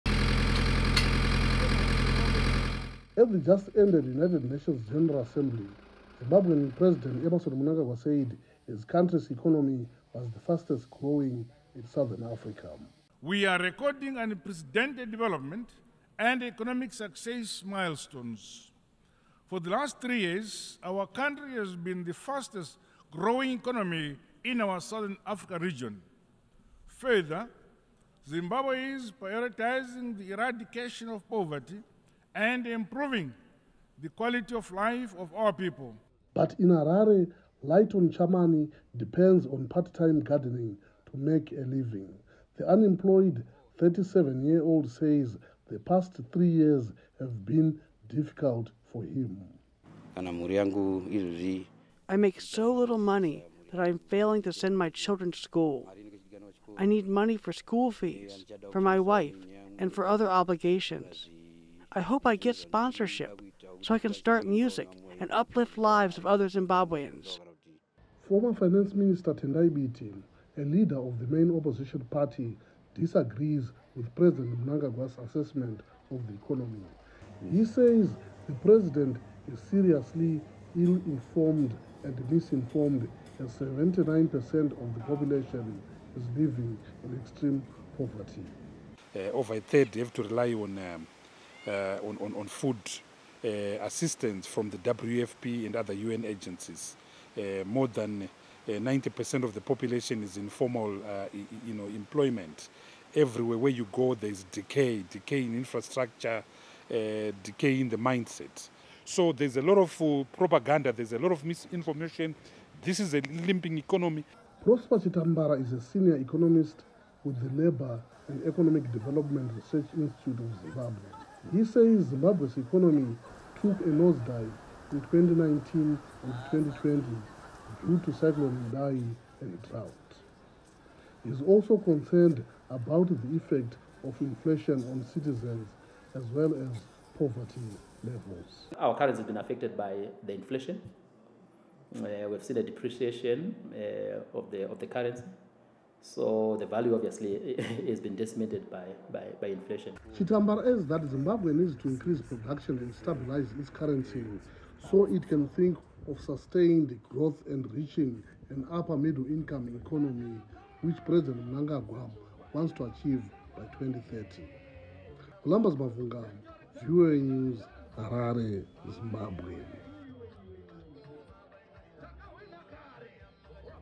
Zimbabwe Mnangagwa Economy USAGM.mp4
Some Zimbabweans living in abject poverty are reacting angrily to claims by President Mnangagwa that the country’s economy is the fastest growing in the southern African region. Economists and members of the main opposition say the president does not understand basic economics.